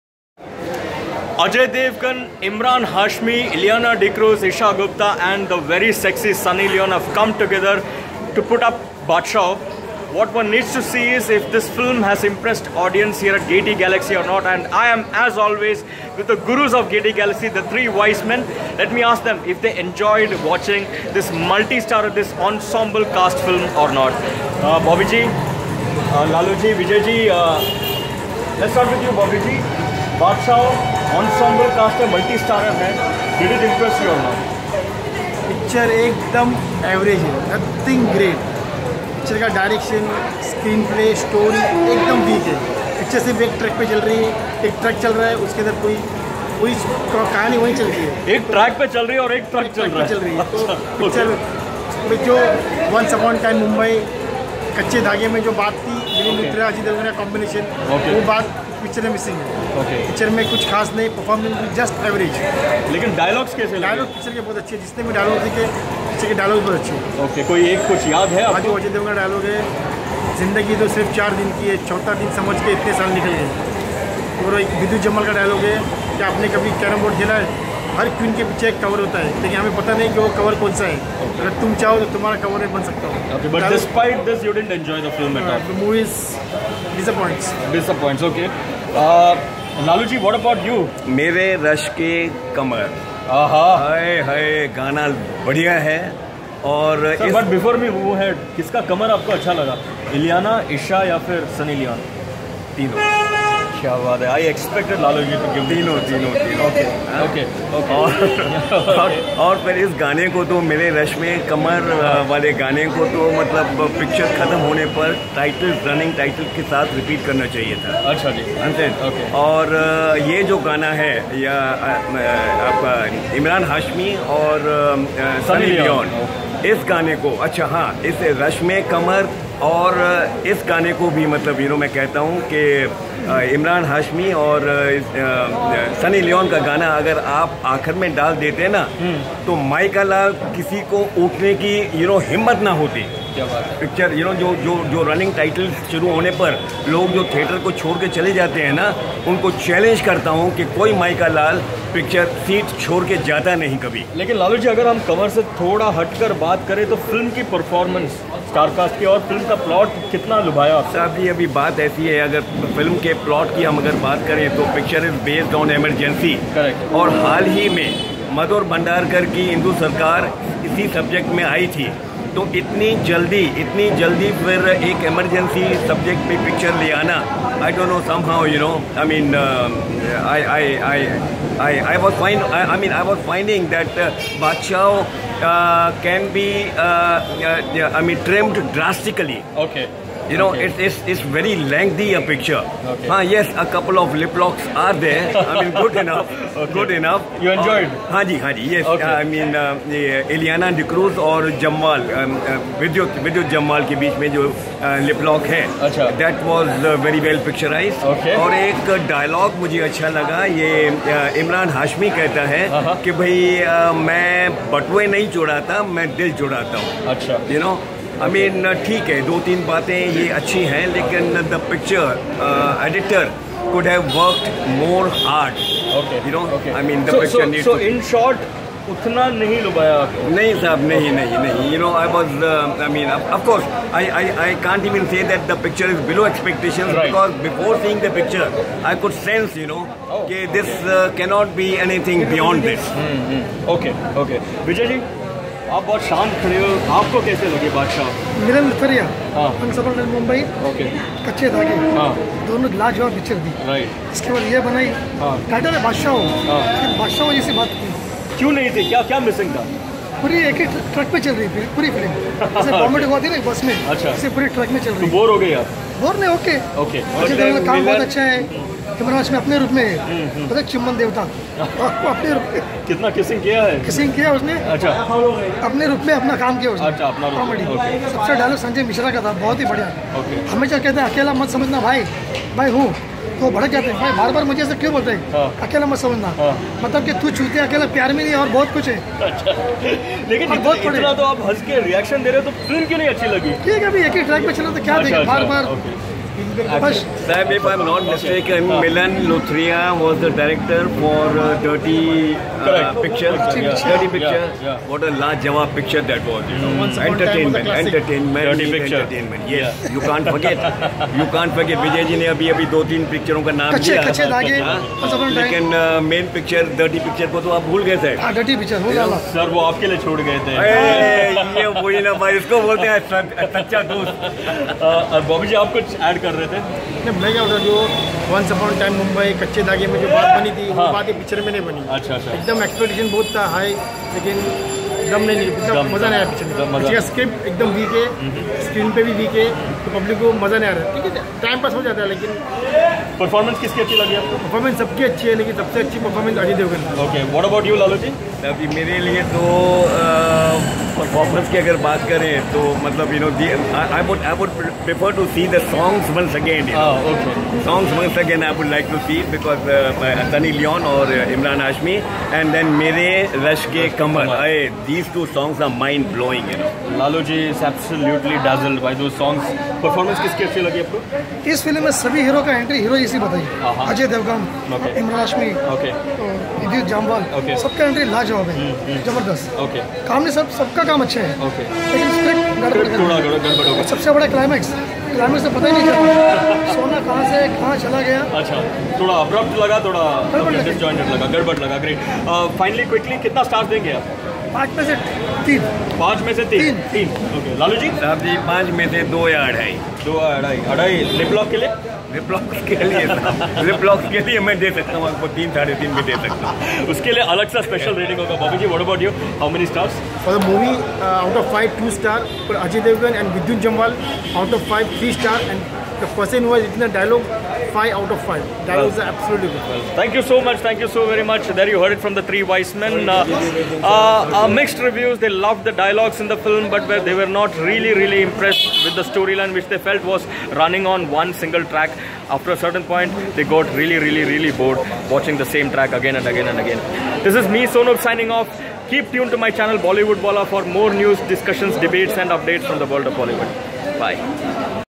Baadshaho public review - Hit or Flop?
The three wise men were disappointed after watching Baadshaho and called it an average movie.